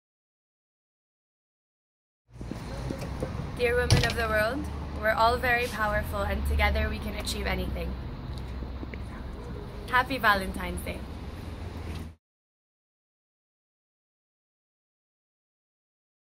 Valentijnsdagboodschap Jordanië 2
Wij vragen tijdens de Valentijnsperiode aandacht voor het brengen van vrede, liefde en solidariteit aan alle mensen die zich in noodsituaties bevinden. Deze indrukwekkende liefdesverhalen laten zien dat liefde overwint, deel ook jouw boodschap of deze verhalen met de wereld via social media met CAREforLove.